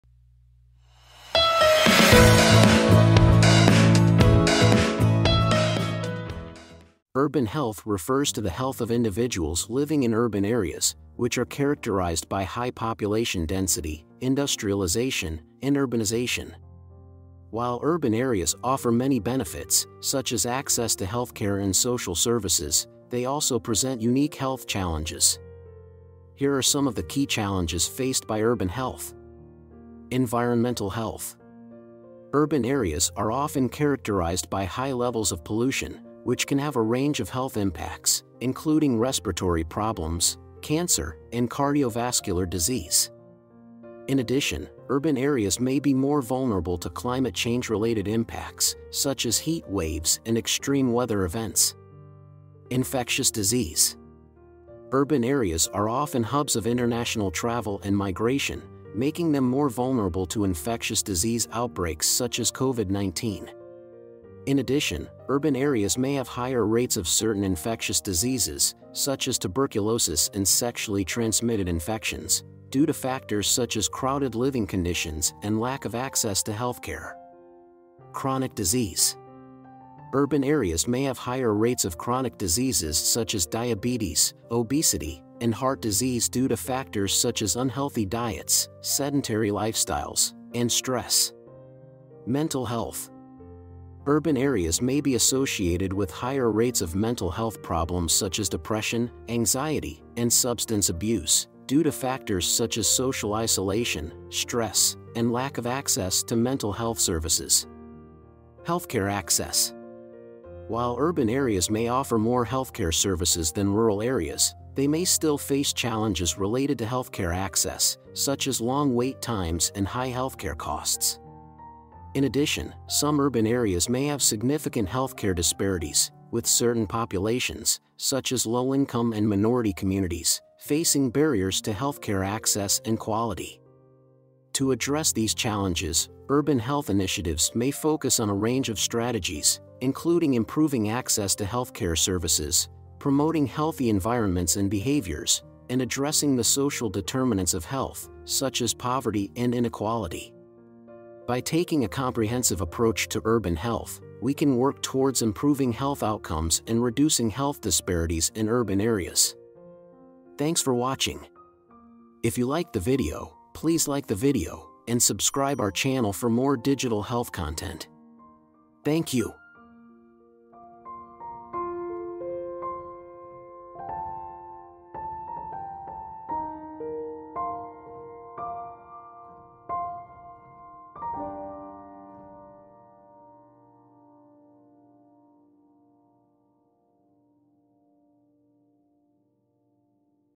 Introduction to Urban Health and its unique Challenges
Urban health refers to the health of individuals living in urban areas, which are characterized by high population density, industrialization, and urbanization. While urban areas offer many benefits, such as access to healthcare and social services, they also present unique health challenges.
To address these challenges, urban health initiatives may focus on a range of strategies, including improving access to healthcare services, promoting healthy environments and behaviors, and addressing the social determinants of health, such as poverty and inequality. By taking a comprehensive approach to urban health, we can work towards improving health outcomes and reducing health disparities in urban areas.